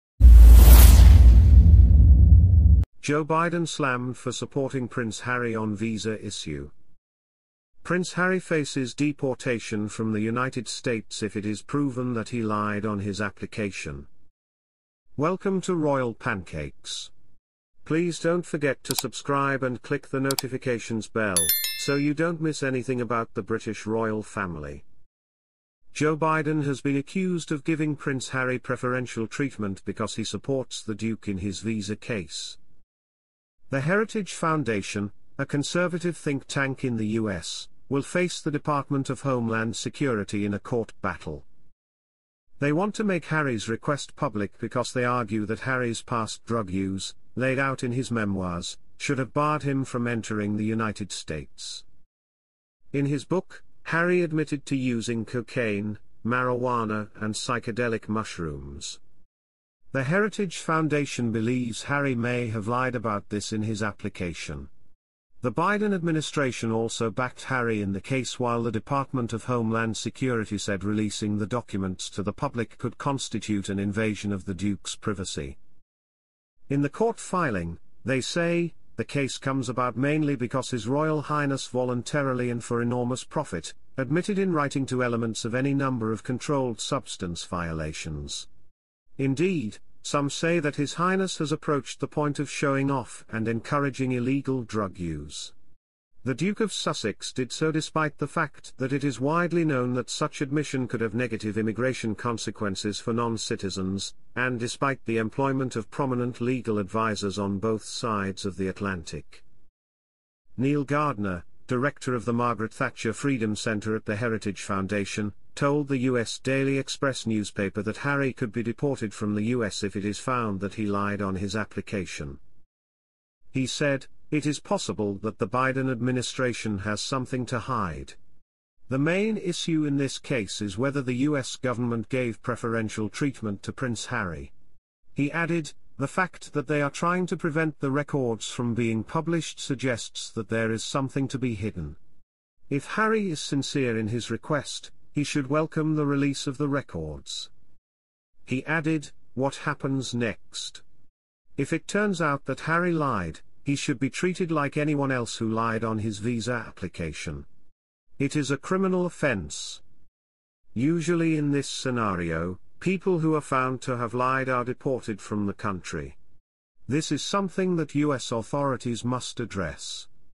REVENGE!🛑 Angry Harry plants Massive Bomb in the white house after Joe Biden deports him from the US
Joe Biden slammed for supporting Prince Harry on visa issue.

Prince Harry faces deportation from the United States if it is proven that he lied on his application.

Joe Biden has been accused of giving Prince Harry preferential treatment because he supports the Duke in his visa case.

The Heritage Foundation, a conservative think tank in the US, will face the Department of Homeland Security in a court battle.

They want to make Harry's request public because they argue that Harry's past drug use, laid out in his memoirs, should have barred him from entering the United States.

In his book, Harry admitted to using cocaine, marijuana and psychedelic mushrooms.
The Heritage Foundation believes Harry may have lied about this in his application. The Biden administration also backed Harry in the case while the Department of Homeland Security said releasing the documents to the public could constitute an invasion of the duke's privacy.

In the court filing, they say: “The case comes about mainly because His Royal Highness voluntarily and for enormous profit — admitted in writing to elements of any number of controlled substance violations. Indeed, some say that His Highness has approached the point of showing off and encouraging illegal drug use. The Duke of Sussex did so despite the fact that it is widely known that such admission could have negative immigration consequences for non-citizens, and despite the employment of prominent legal advisers on both sides of the Atlantic.”

Niall Gardiner, director of the Margaret Thatcher Freedom Center at the Heritage Foundation, told the US Daily Express newspaper that Harry could be deported from the US if it is found that he lied on his application.

He said: “It is possible that the Biden administration has something to hide. The main issue in this case is whether the US government gave preferential treatment to Prince Harry".
He added: "The fact that they are trying to prevent the records from being published suggests that there is something to be hidden. If Harry is sincere in his request, he should welcome the release of the records."
He added: "What happens next? If it turns out that Harry lied, he should be treated like anyone else who lied on his visa application. It is a criminal offence."
"Usually in this scenario, people who are found to have lied are deported from the country. This is something that US authorities must address."princeharry